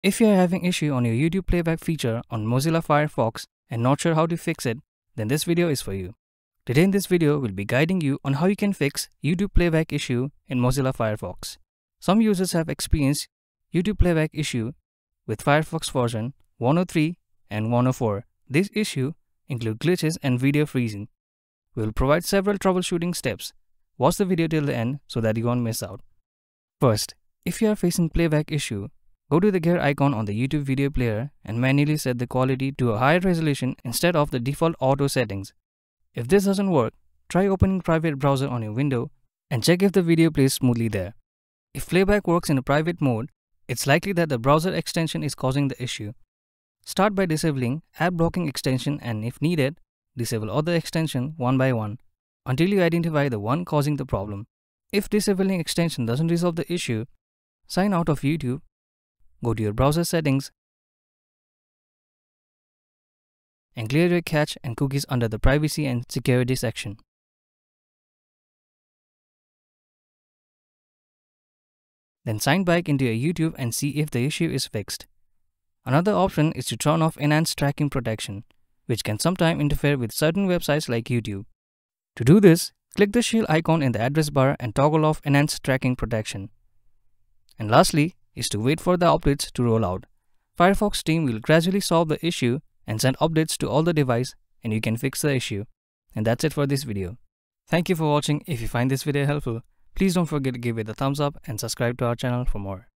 How to Fix YouTube Playback Issues in Mozilla Firefox
Learn how to fix YouTube playback issues in Mozilla Firefox with this comprehensive troubleshooting guide! This video will walk you through steps to resolve buffering, video loading, and playback problems on YouTube when using the Firefox browser. Whether you're asking, "How do I fix YouTube playback issues in Firefox?" or need help tweaking your browser settings, this tutorial has you covered. Watch now to get YouTube running smoothly on Firefox!

Timestamps:

00:00 How to Fix YouTube Playback Issues in Mozilla Firefox
00:10 Troubleshooting YouTube Playback Problems in Firefox